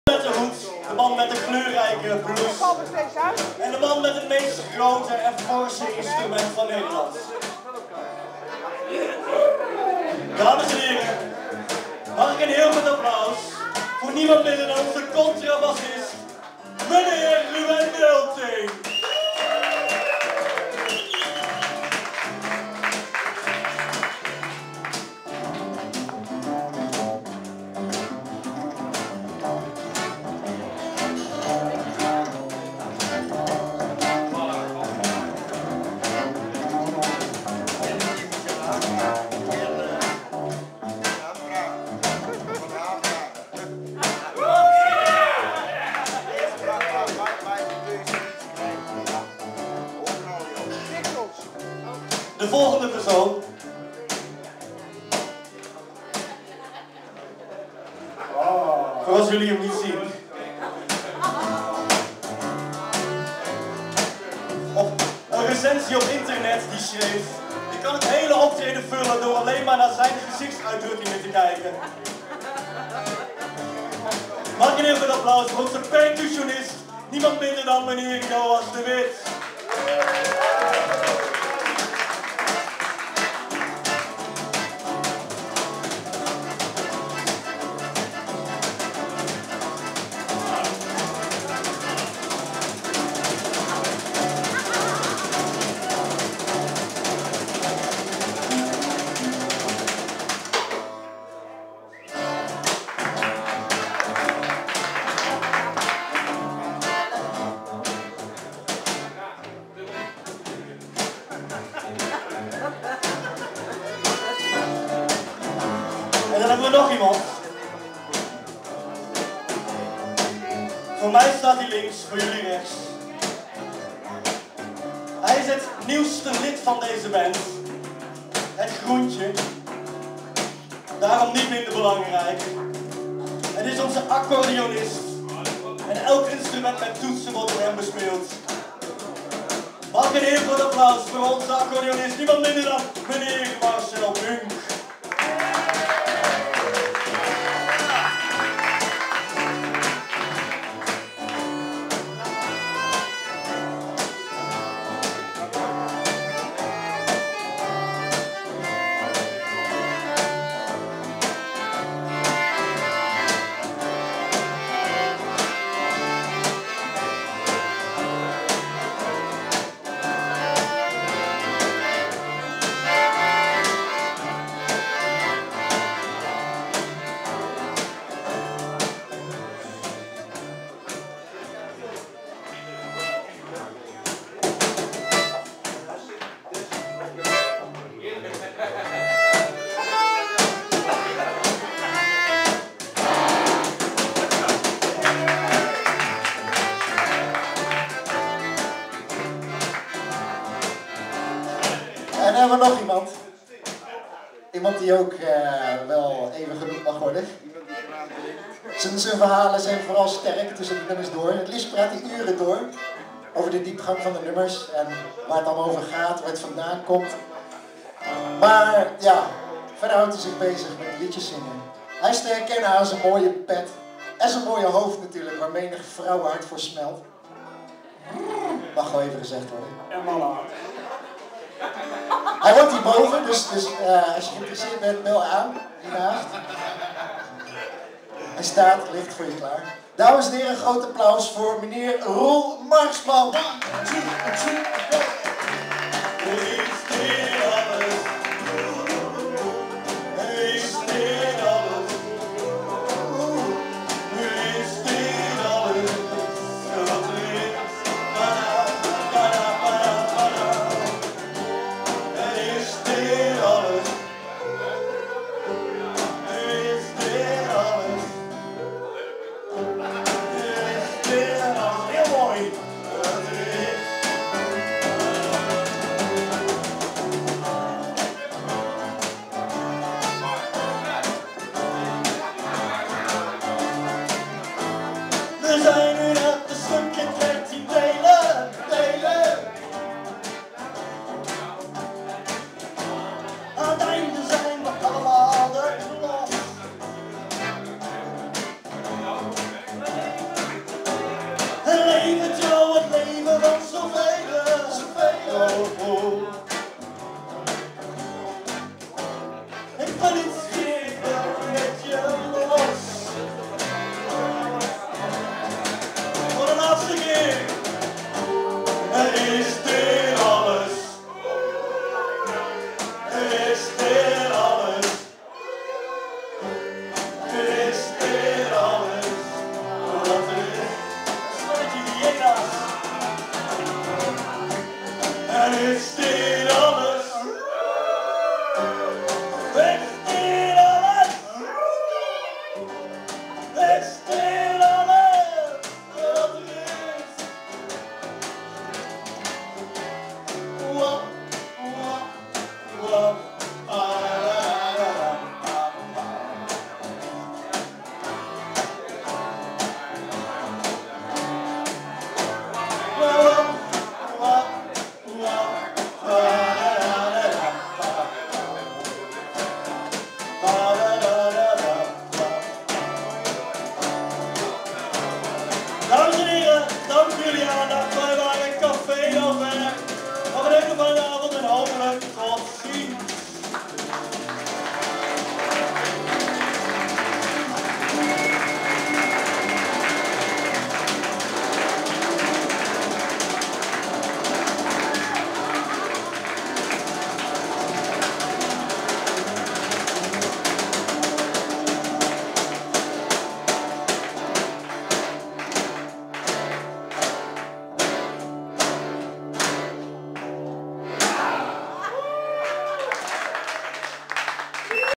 Café d'Anvers
Optreden in cafe Atlanta te Arnhem 12 juli 2009